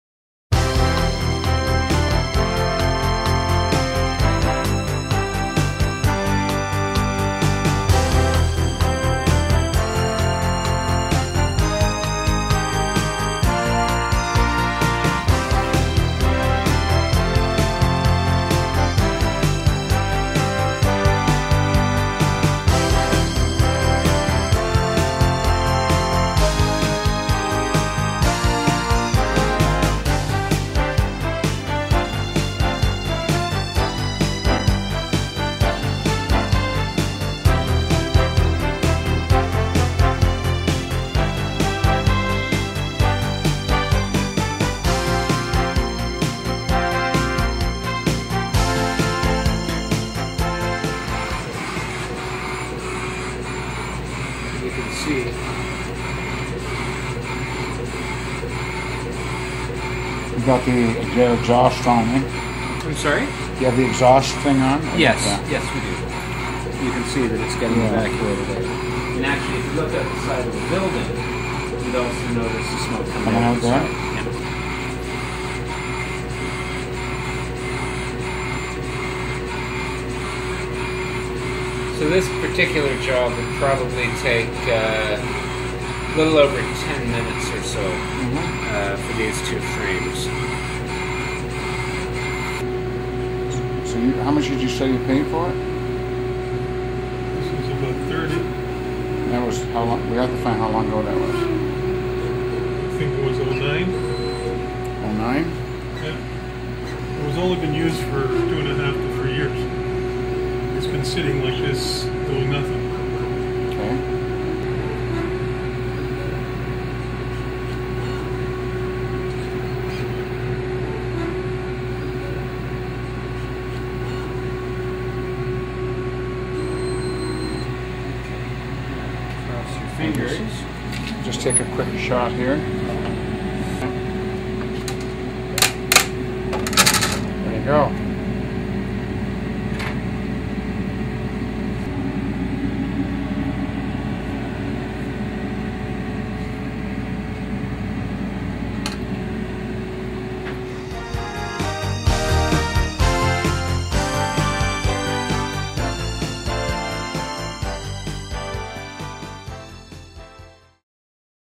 Universal Professional CO2 laser Cutter Engraver 60 Watts for Sale
Universal 24" x 18 process area - 60 Watt Power -Model PLS4.60, 2009 – Lightly used. CDN$ 14,500.  In excellent running condition. Laser tube running up to spec. The PLS4.75 is ideal for processing a wide variety of materials plastic, wood, rubber, acrylic, glass.
Laser Material Processing Area (W x H) 24 inches x 18 inches
Maximum Part Size (W x H x D) 29 x 23 x 9 in. Overall Dimension (W x H x D) 36 x 39 x 36 in.
Comes with Universal Control Software - Laser Platform Interface Panel - Keypad and LCD display shows current file name, laser power, engraving speed, PPI and run time.
Power Requirements: 110V/10A
220V-240V/5A –
Laminated Safety Glass
Over-temp Alarm (for fire safety)
Multiple Automatic Focusing Modes
Precision Digital Motors
LCD display
Multiple Language Support
Permanently Sealed Bearings
Proportional Pulse Control
Stretch-Free Kevlar® Belts
Located in Toronto - Under power for inspection. Call Jim for demo: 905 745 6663